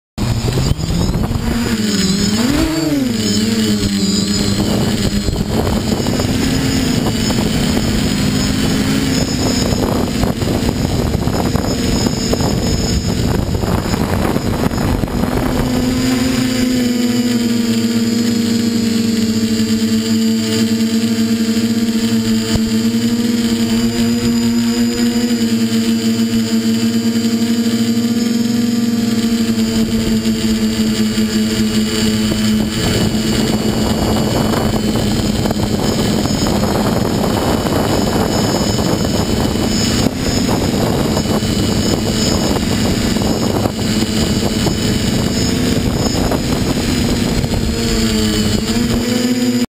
torxpower 900kv v2
sorry for the brief video guys forgot to charge go pro battery ): was a quick test of my new torxpower 900kv v2 210w motors bags of power but i was having issues with my ezuhf rx witch i didnt manage to record jello has almost gone niceand ballanced motors